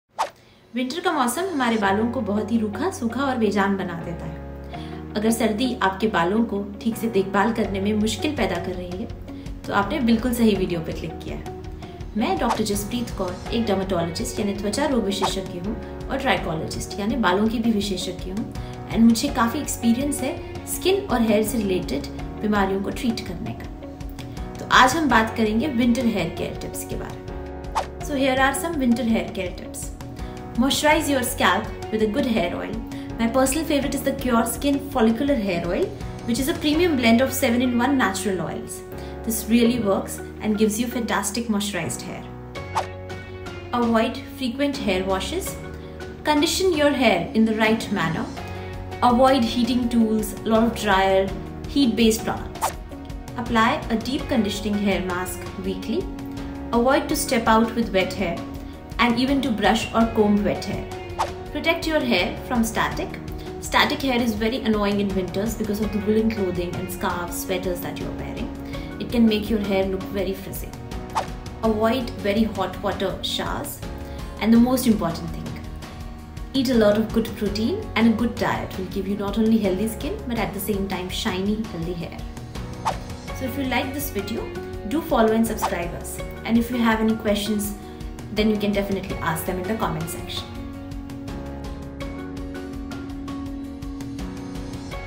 In tips ke saath paayein winter me bhi swasth baal - Dr. Jaspreet Kour | CureSkin
Winter ka mausam hamari skin ke saath-saath lagbhag hamaare baalon ko bhi rookha aur bejaan bana deta hai, hai na? Isliye sardiyon mein na keval humein apni skin ki balki uske saath hi baalon ki bhi extra care karne ki zaroorat hoti hai.

To aaj Dr Jaspreet Kour ,unki expert tips ke saath aapki madad ke liye yahan hain. Jo aapko batayengi ki aap kaise in sardiyon mein rookhe aur bejaan baalon se bach sakte hain aur kaise kuch aasaan si tips ke saath sardiyon mein bhi healthy hair paa sakte hain.

Timestamp:
00:00 Intro
00:27 Hair care tips for winter
01:23 Conclusion